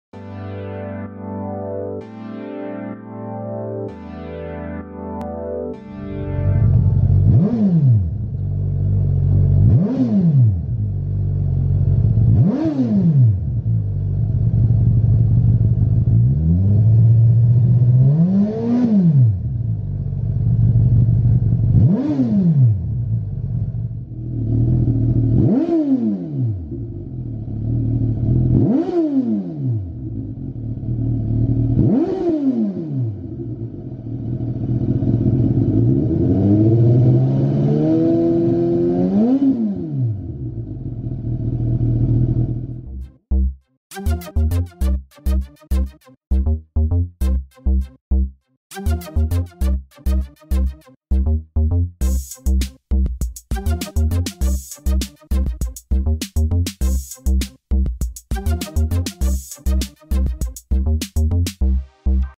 Lextek CP1 Exhaust Sound: Suzuki GSX-S1000 (15-18)
This video is to demonstrate the exhaust sound of the Lextek CP1 Silencer. The bike this was fitted to was a Suzuki GSX-S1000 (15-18).